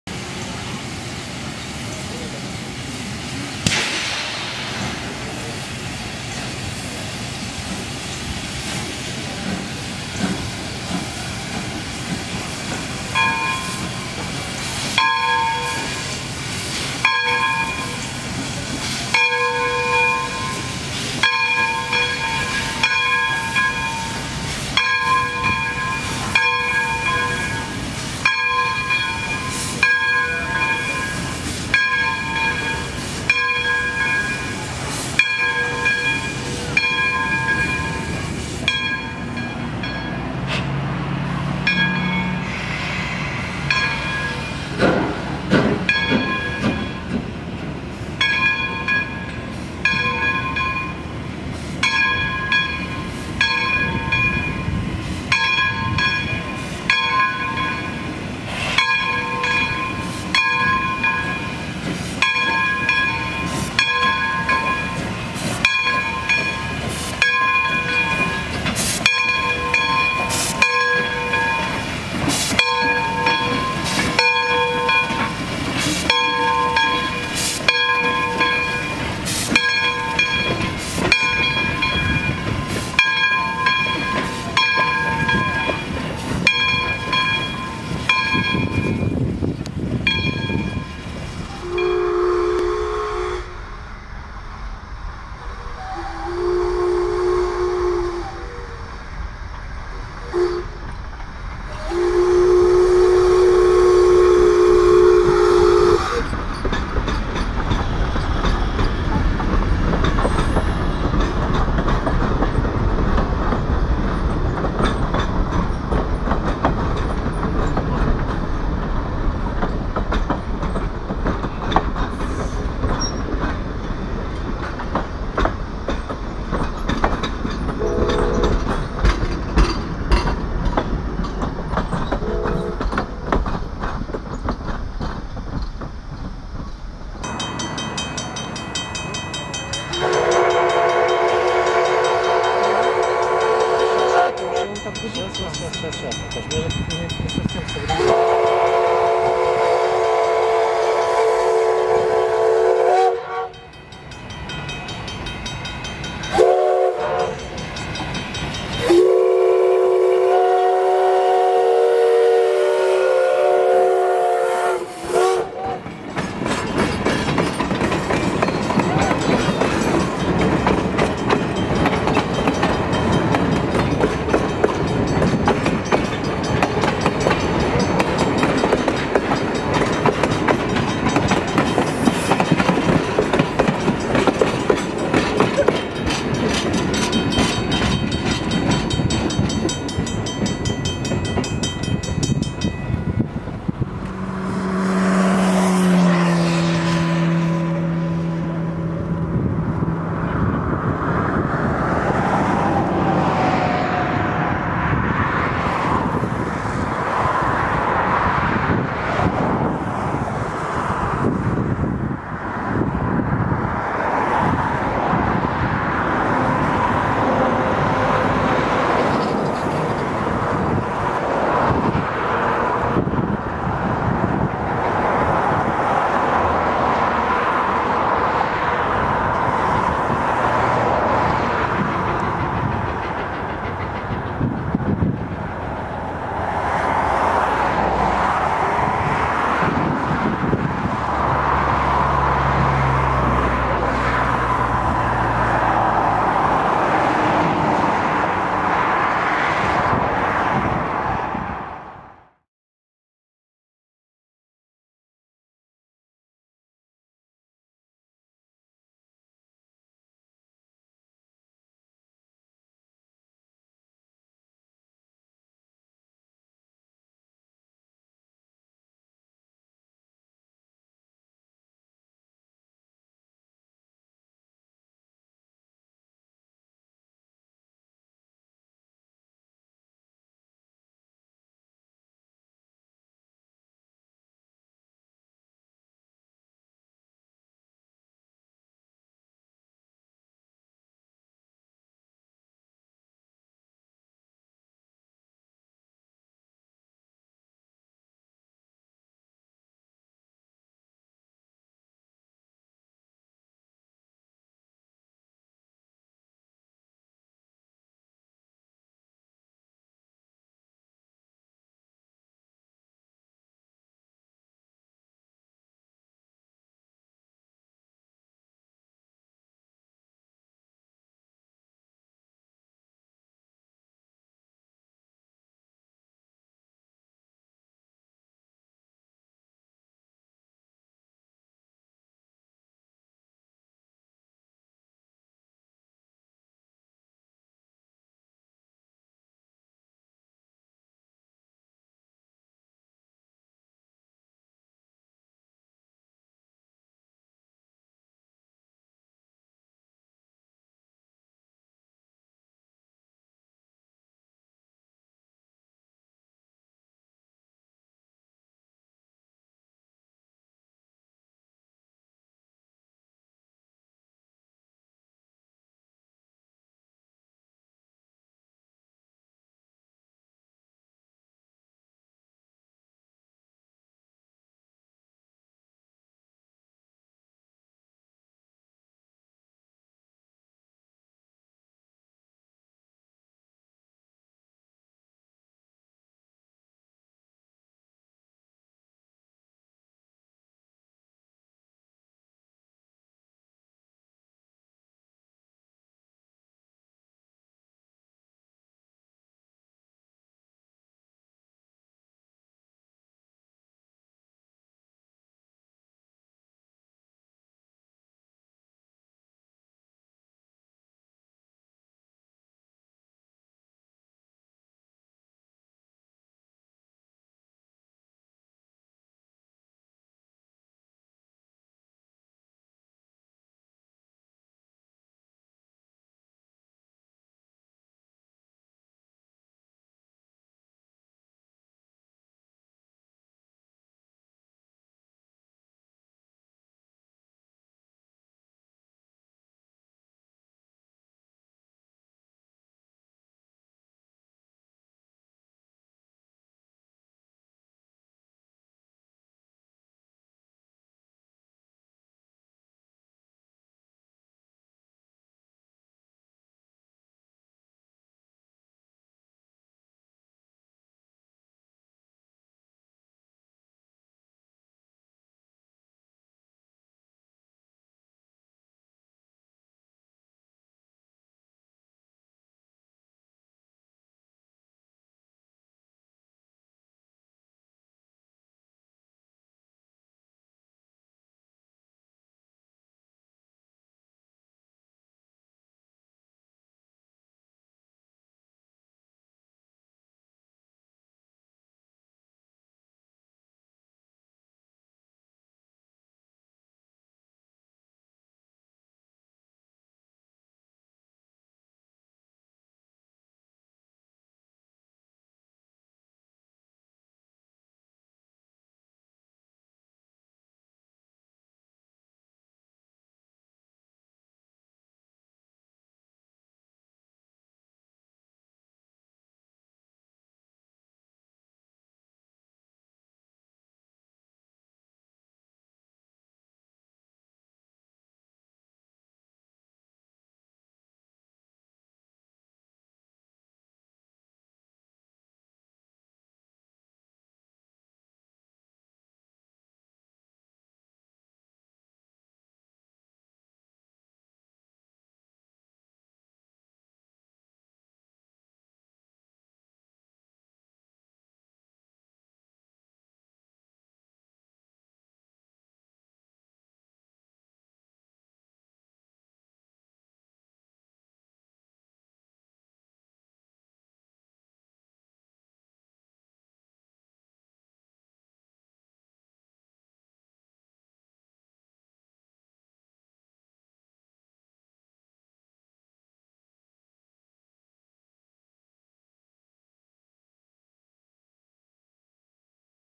Waterloo Central Railway steam excursion, Elmira to Waterloo [drone] (23-May-2016)
Waterloo Central Railway steam excursion, southbound from Elmira to Waterloo Station, including photo run-by (x2) at Scotch Line Road halfway between St Jacobs and Elmira.

Essex Terminal Railway Company #9 "Spirit of Elgin" 0-6-0 steam locomotive
Waterloo Central Railway caboose #1040 (yellow)
Waterloo Central Railway #1437 "Midway" passenger coach (Canadian Pacific maroon)
Waterloo Central Railway #3216 passenger coach (VIA blue/yellow)
Waterloo Central Railway #5504 passenger coach (VIA blue/yellow)
Waterloo Central Railway caboose #61 (red)

Note: the drone doesn't record audio so the last half is silent.